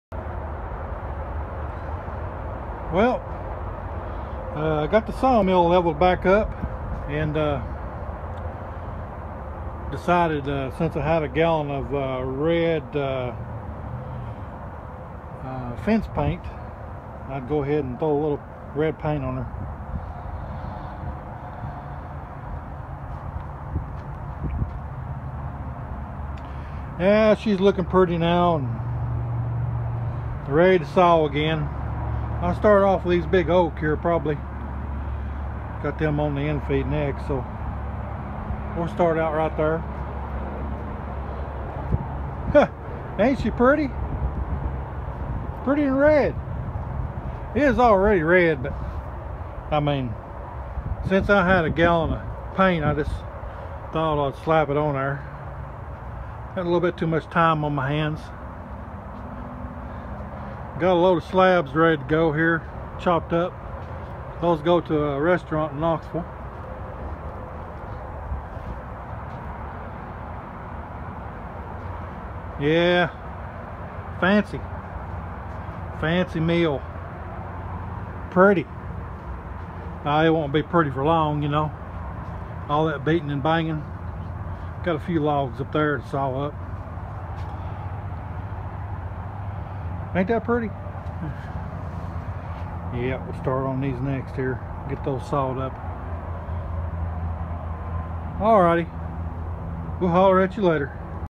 The Foley Belsaw sawmill; Red fence paint. ( humorous)
After some repairs and adjustments, I slapped on some fresh red paint. I reckon I was bored.